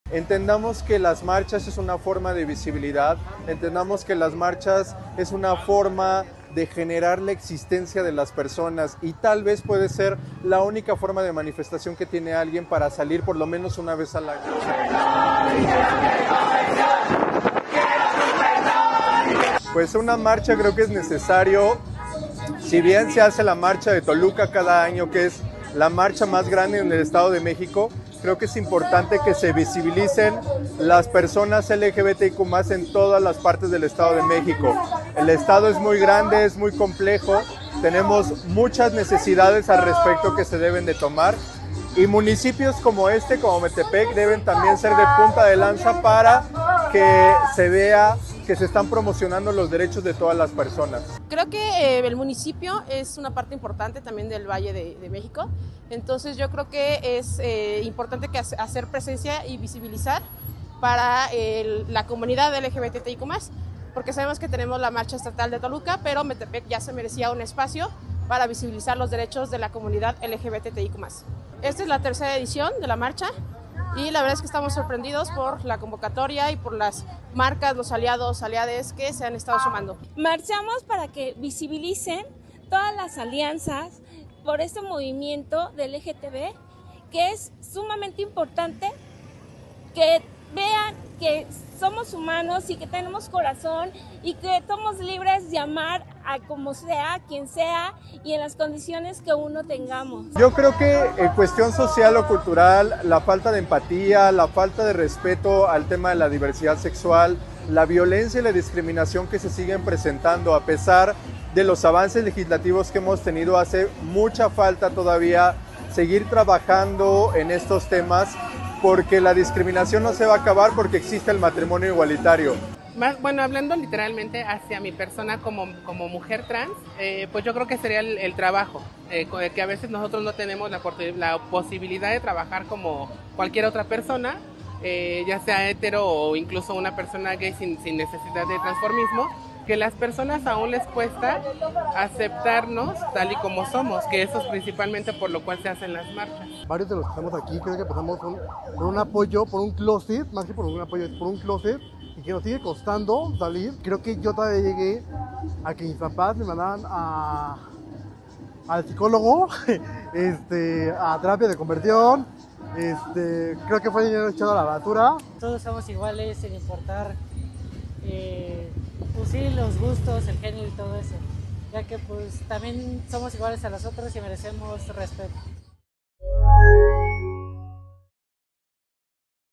La comunidad LGBT tomó las calles de Metepec
El centro de Metepec se llenó de colores este sábado 8 de junio. La Marcha del Orgullo, organizada por la comunidad LGBTTTIQ+ recorrió las calles del primer cuadro.